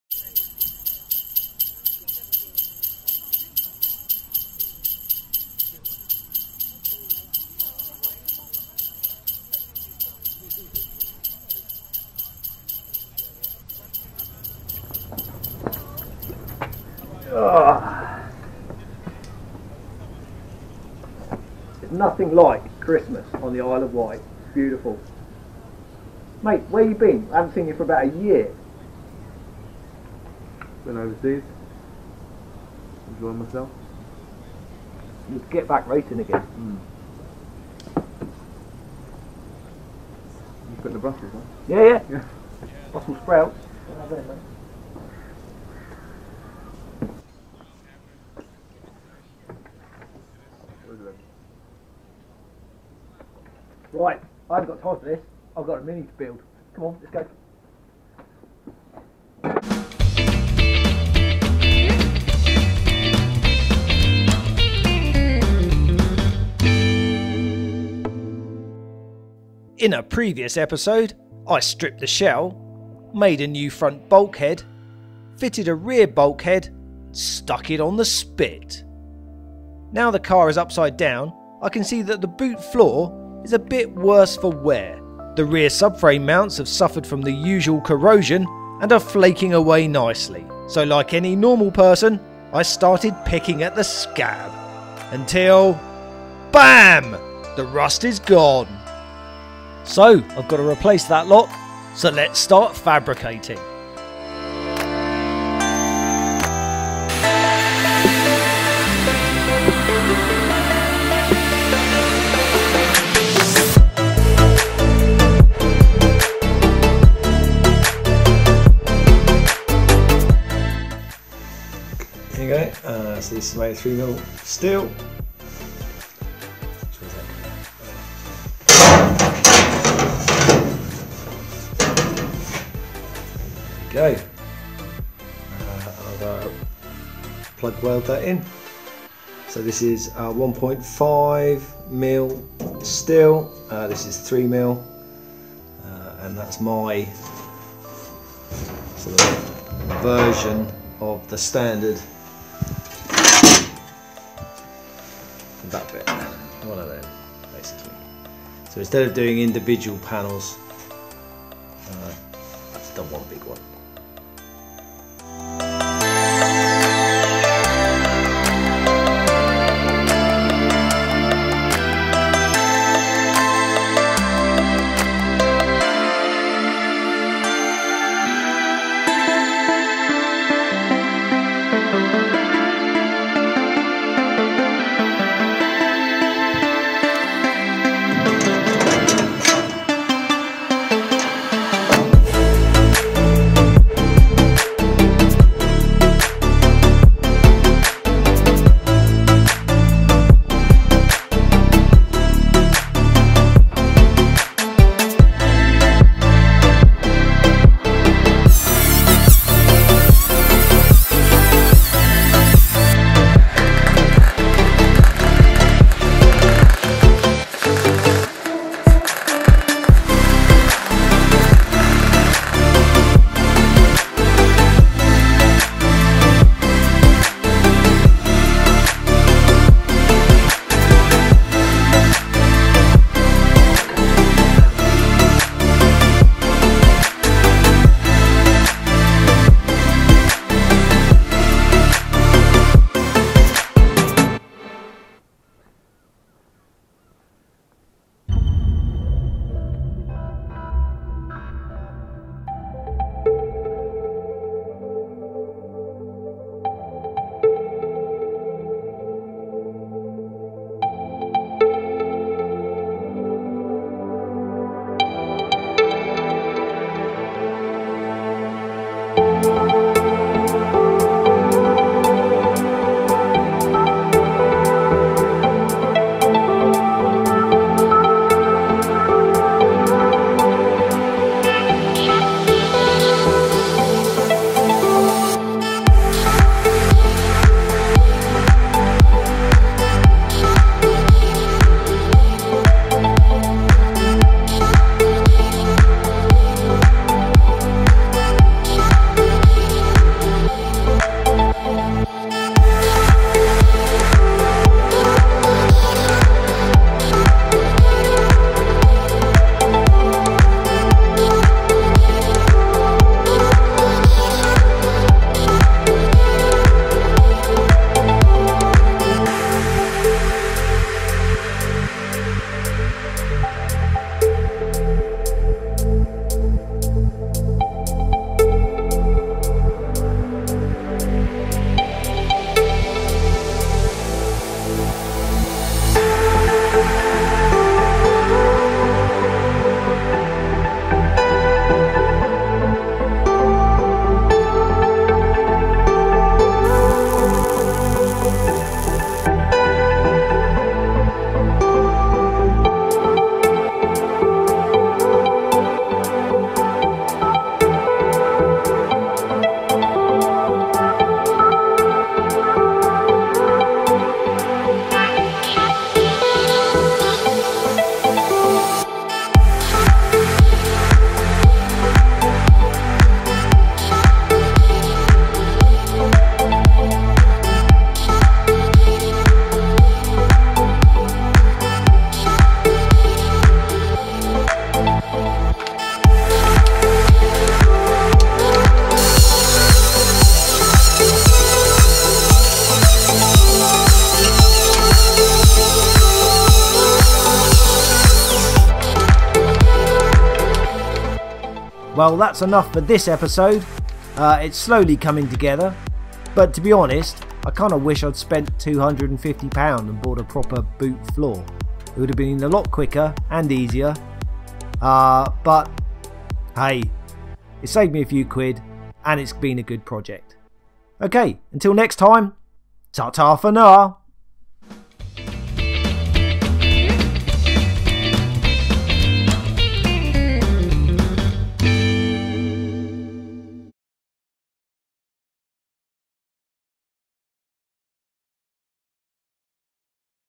Building a rally classic mini part 2
In this episode we discover a very rotten boot floor so we fabricate a new one.

We now have a GoFundMe page if you wish to support our campaign to rally this classic mini - in particular we hope to crowd fund the regulation fire extinguishers needed to compete in national and international stage rally's, if you can help, be part of the team and head to our GoFundMe page.